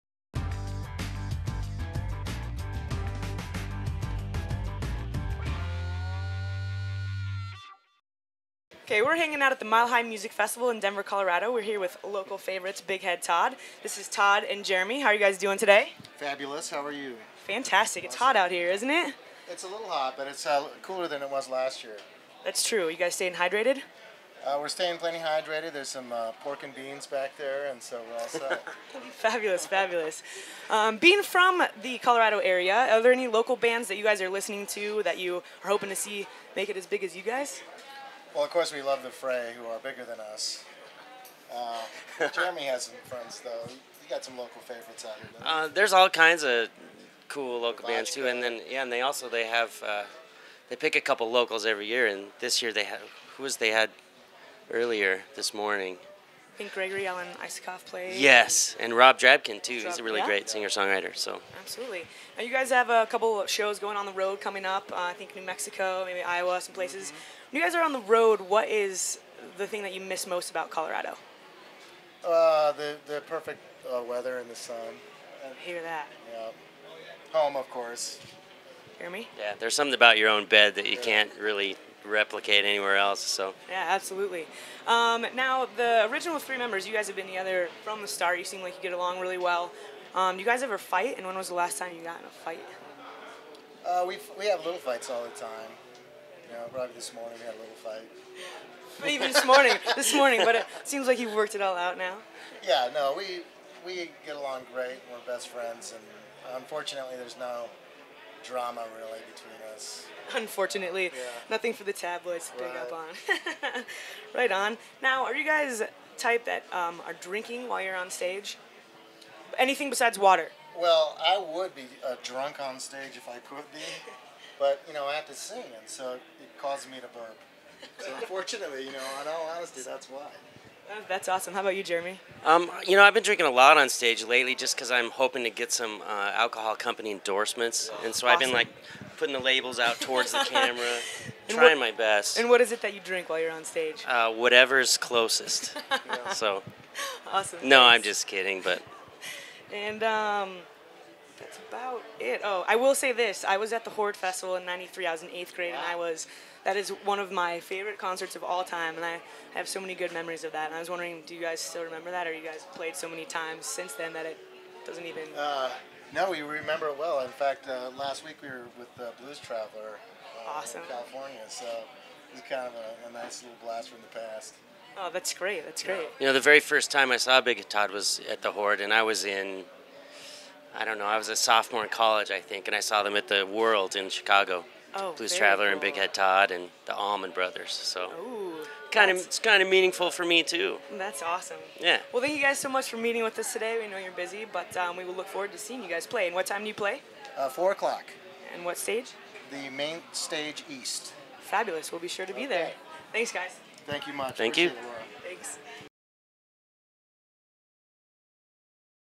Big Head Todd interview Mile High Music Festival for Examiner.com
Big Head Todd from Big Head Todd and the Monsters with Denver Music Examiner Laura McDonald at the 2009 Mile High Music Festival.